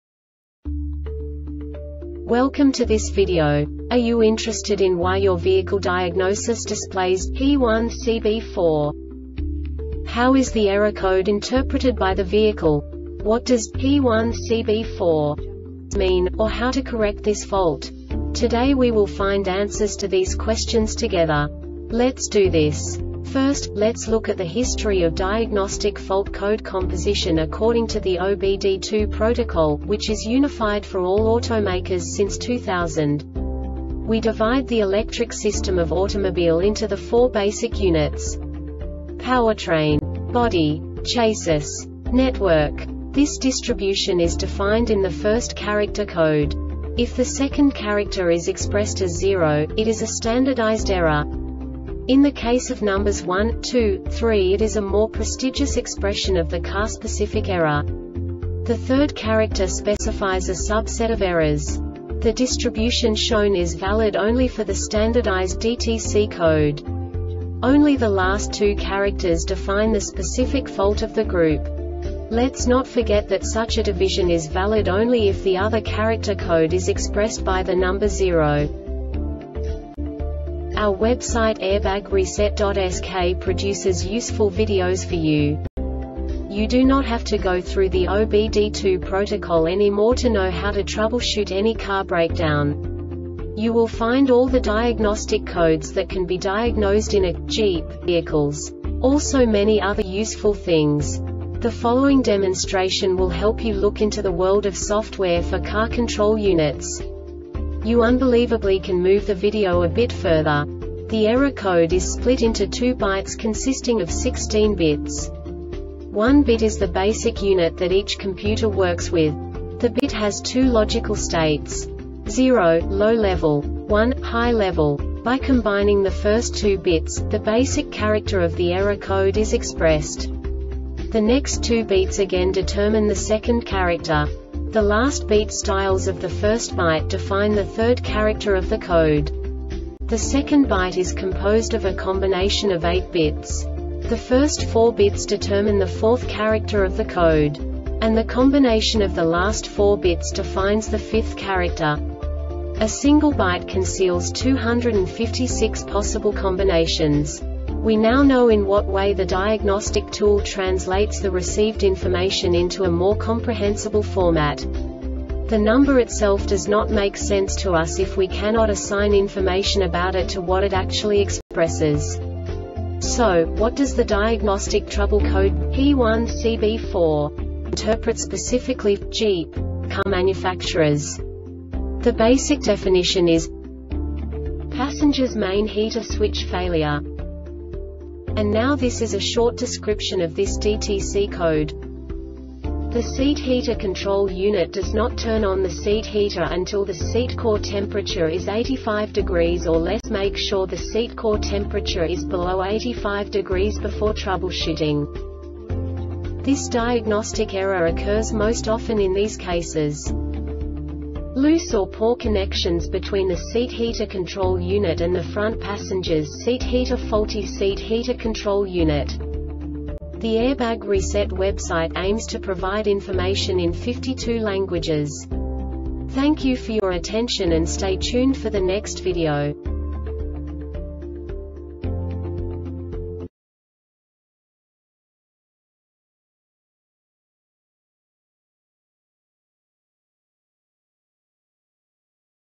DTC Jeep P1CB4 Short Explanation
Contents:
0:21 Basic DTC analysis according to OBD2 protocol standard.
1:48 Insight into programming
2:58 A diagnostically understandable description of the Diagnostic Trouble Code
3:05 Basic error definition

Make
Jeep
Code
P1CB4
Definition
Passenger's Main Heater Switch Failure
Description
The seat heater control unit does not turn on the seat heater until the seat core temperature is 85 degrees or less  Make sure the seat core temperature is below 85 degrees before troubleshooting
Cause
 Loose or poor connections between the seat heater control unit and the front passenger's seat heater Faulty seat heater control unit